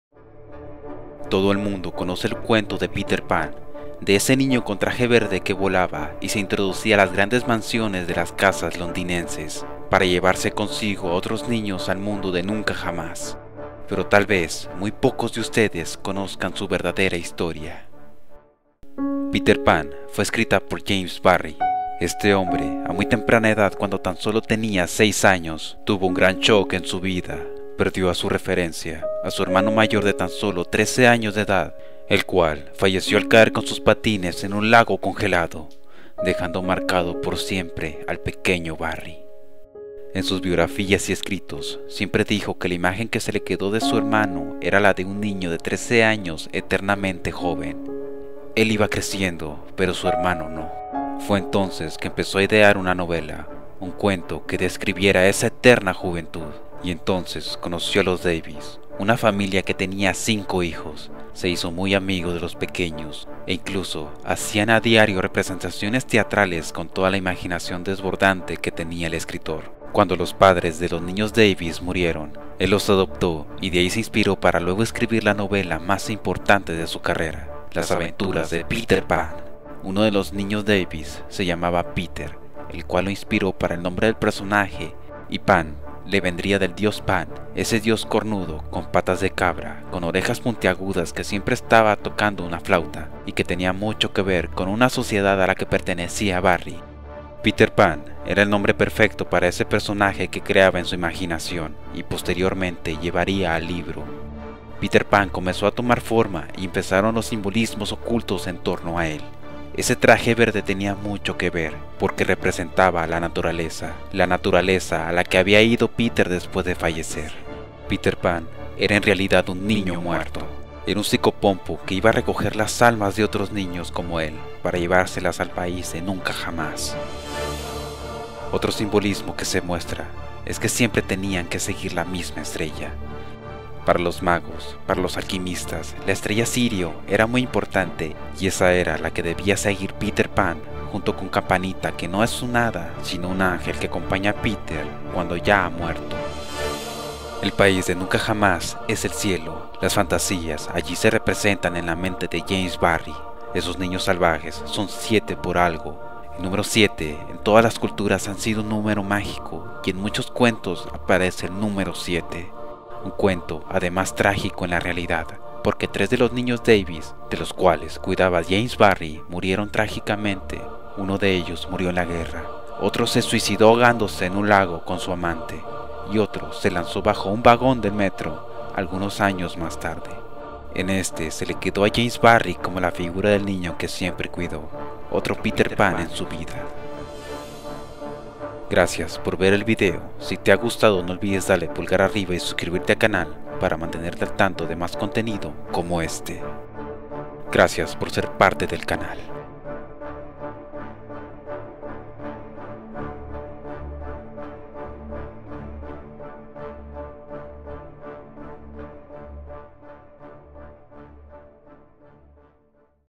Peter Pan: La Verdadera Historia | Rembao
La verdadera historia de Peter Pan fue creada por el novelista británico James Matthew Barrie para una obra de teatro estrenada en Londres el 27 de diciembre de 1904 llamada "Peter Pan y Wendy".